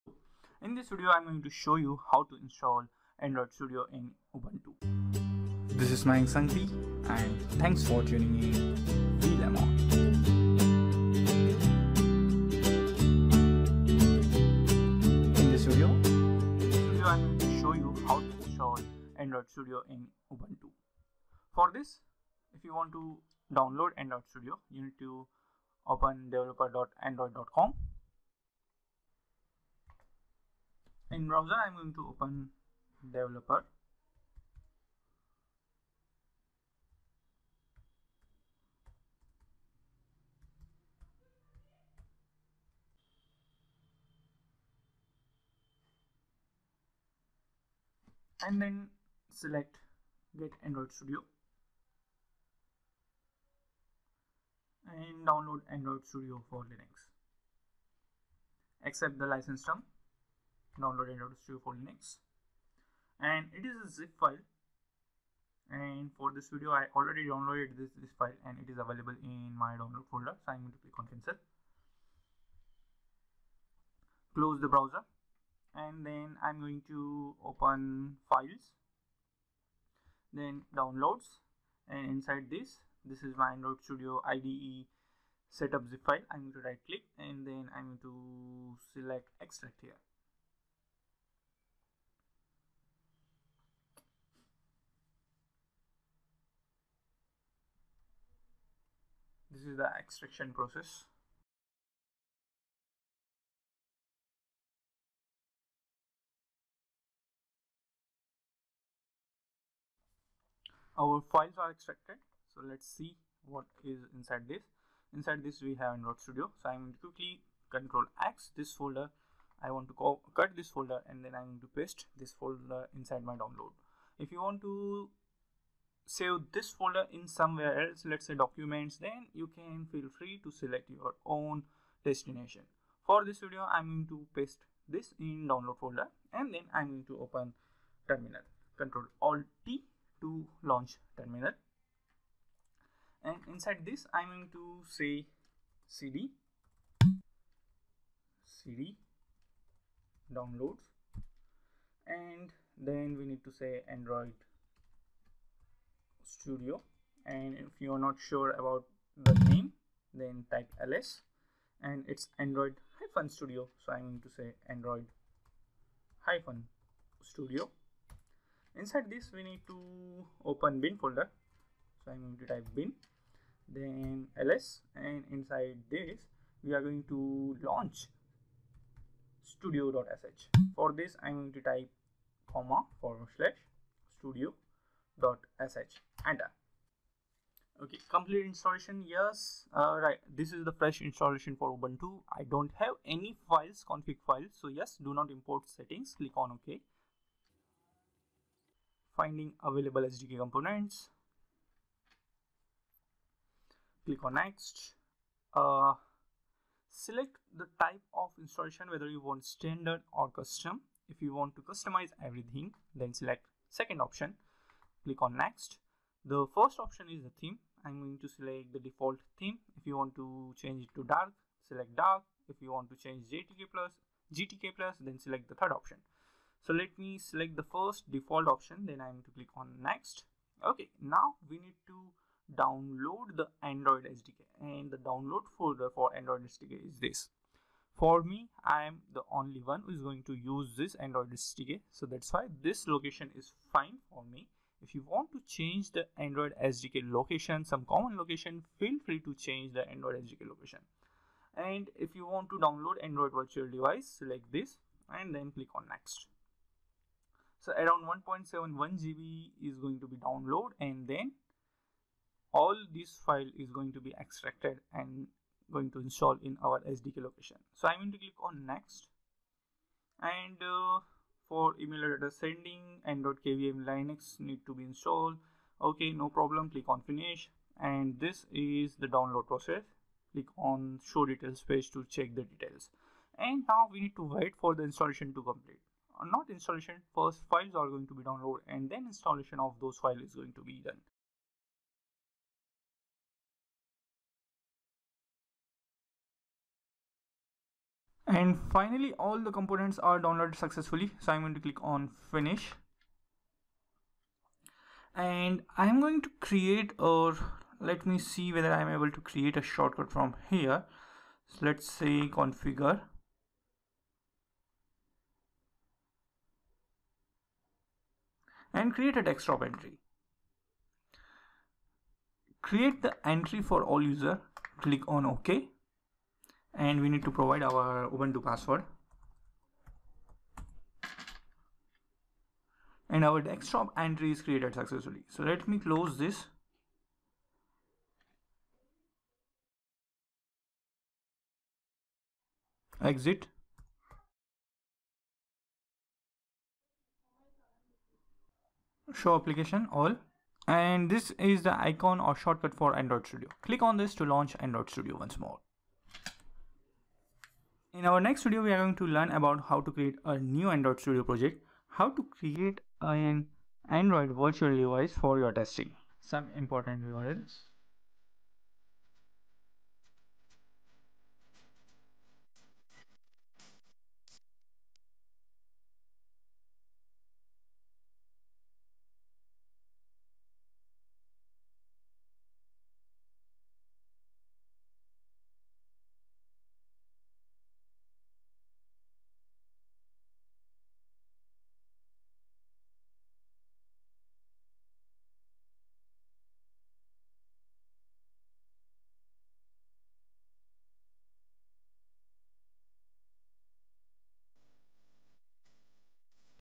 How to Install Android Studio 3.0.1 in Ubuntu 17.10
Install Android Studio in Ubuntu 17.10

In this video, we are going to download and install Android Studio in Ubuntu 17.10

It helps you to answer
- How to visit Android Studio Office download Website
- How to download Android Studio
- How to install Android Studio in Linux
- How to install required SDK components

This video is the first step in our other tutorial video.
Check the following tutorial series related to this video
1. Android App Development
2. Android App Development with Kotlin Programming Language